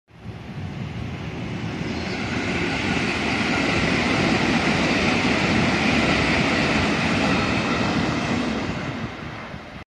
313系(下り)JR相見駅周辺通過
公園でちびっこと一緒に遊んでいる時に撮影してみました･･･